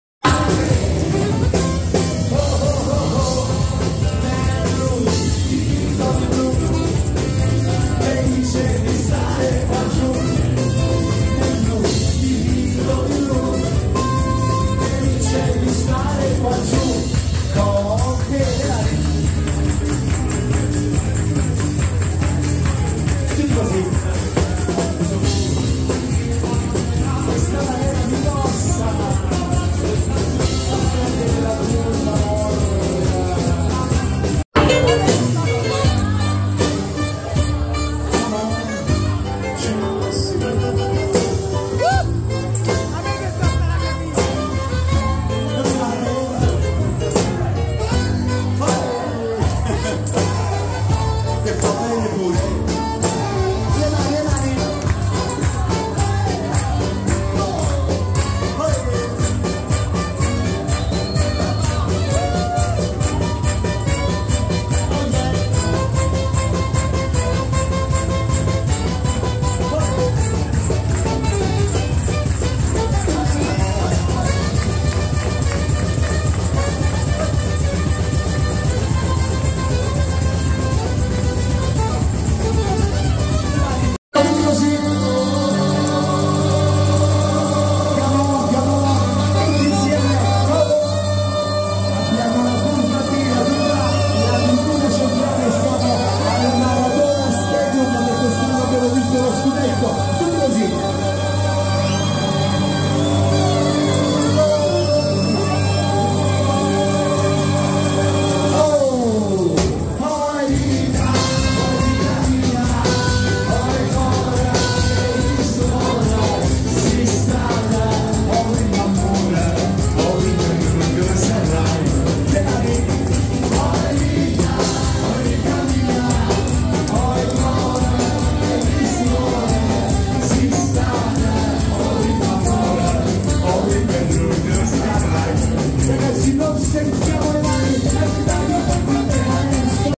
Chiara Ferragni scatenata a Capri con Chiara Biasi
Notte caprese alla taverna Anema e Core per Chiara Ferragni senza Fedez. 11 giugno 2023